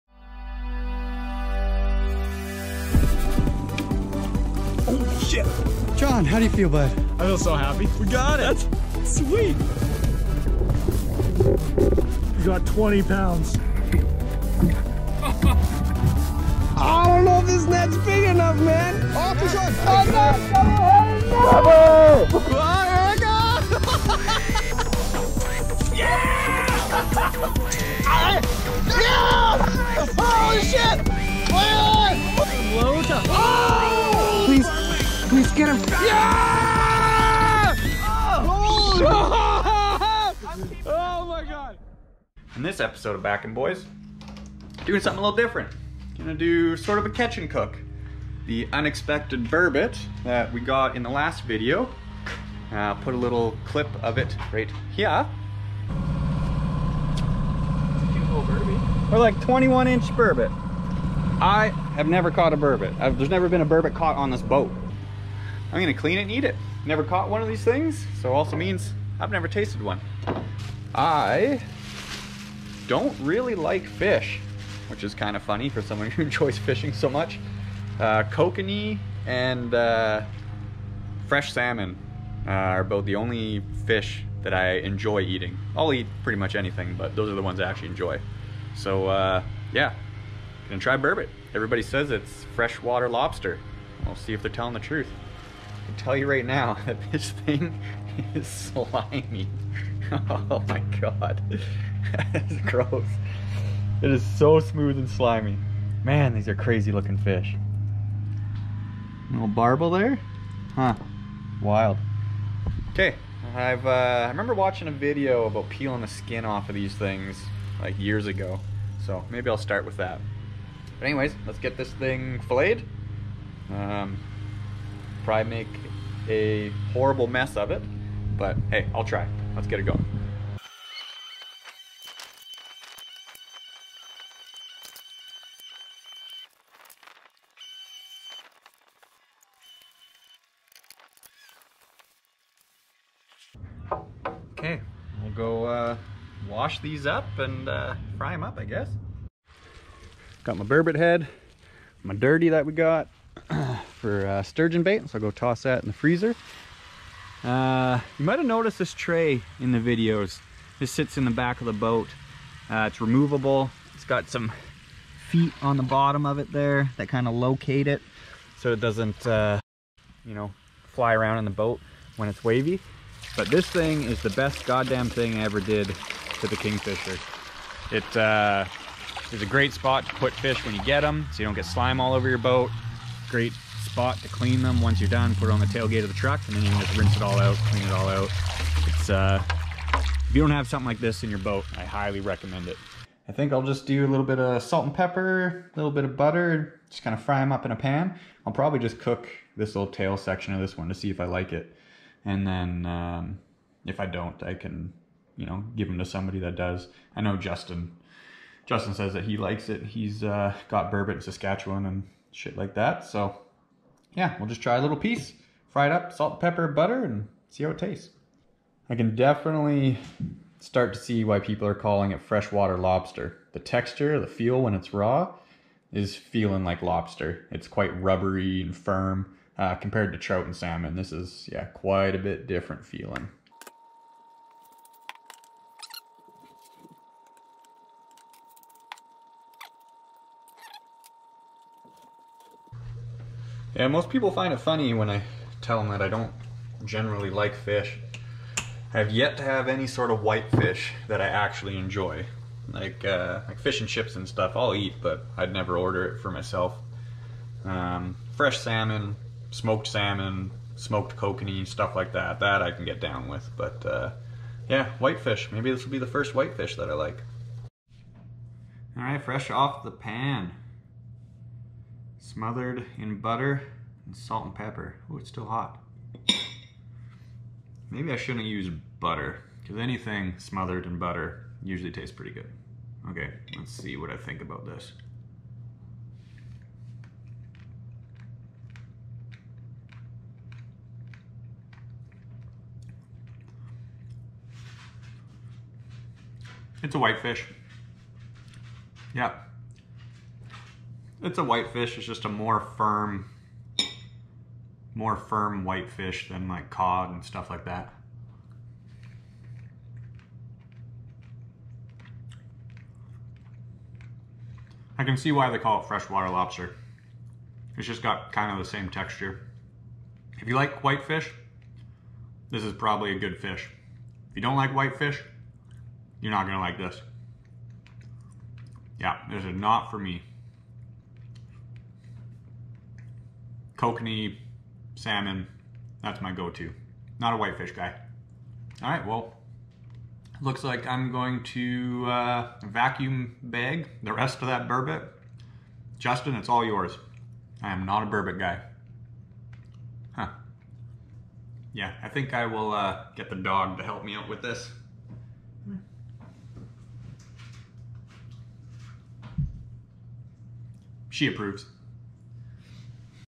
Burbot catch and cook. First time eating burbot.
not a fan.